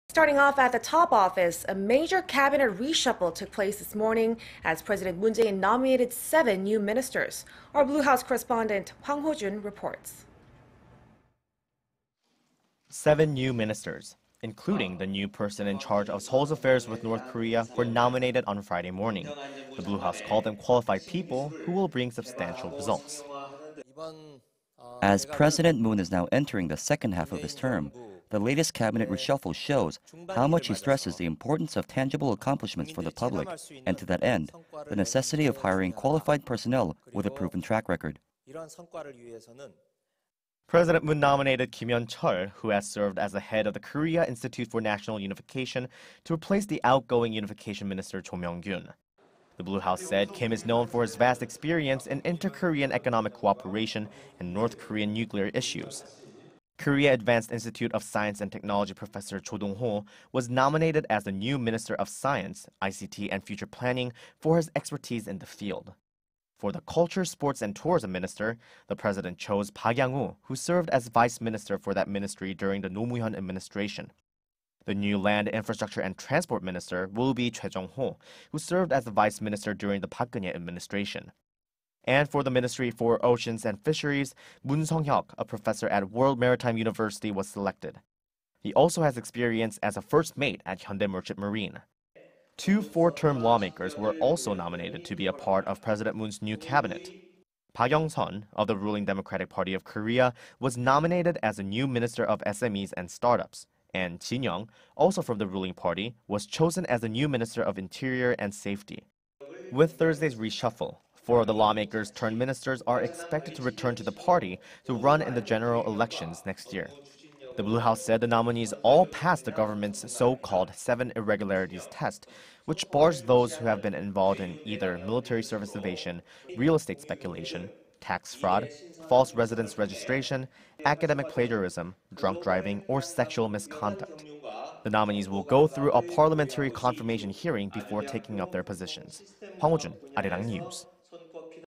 President Moon Jae-in nominates seven new ministers to join Cabinet
청와대, 통일부 포함한 7개부처 중폭 개각

Starting off at the top office,... a major cabinet reshuffle took place this morning as President Moon Jae-in nominated seven new ministers.
Our Blue House correspondent Hwang Hojun reports.
 Seven new ministers,... including the new person in charge of Seoul's affairs with North Korea were appointed on Friday morning; the Blue House called them qualified people who will bring substantial results.
"As President Moon is now entering the second half of his term, the latest cabinet reshuffle shows how much he stresses the importance of tangible accomplishments for the public,...and to that end,... the necessity of hiring qualified personnel with a proven track record."
President Moon nominated Kim Yeon-chul  , who has served as the head of the Korea Institute for National Unification, to replacing the outgoing Unification Minister Cho Myoung-gyon.
The Blue House said Kim is known for his vast experience in inter-Korean economic cooperation and North Korean nuclear issues.
Korea Advanced Institute of Science and Technology Professor Cho Dong-ho   was nominated as the new Minister of Science, ICT and Future Planning for his expertise in the field.
For the culture, sports and tourism minister,... the president chose Park Yang-woo  who served as vice-minister for that ministry during the Roh Moo-hyun administration.
The new land, infrastructure and transport minister,... will be Choi Jeong-ho , who served as the vice-minister during the Park Geun-hye administration.
And for the Ministry for Oceans and Fisheries,... Moon Seong-hyeok   a professor at World Maritime University was selected. He also has experience as a first mate at Hyundai Merchant Marine.
Two four-term lawmakers were also nominated to be a part of President Moon's new cabinet.
Park Young-sun of the ruling Democratic Party of Korea was nominated as the new minister of SMEs and Startups,
and   Chin Young, also from the ruling party, was chosen as the new minister of interior and safety.
With Thursday's reshuffle,.. four of the lawmaker-turned-ministers are expected to return to the party to run in the general elections next year.
The Blue House said the nominees all passed the government's so-called seven irregularities test,... which bars those who have been involved in either military service evasion, real estate speculation, tax fraud, false residence registration, academic plagiarism, drunk driving, or sexual misconduct.
The nominees will go through a parliamentary confirmation hearing before taking up their positions.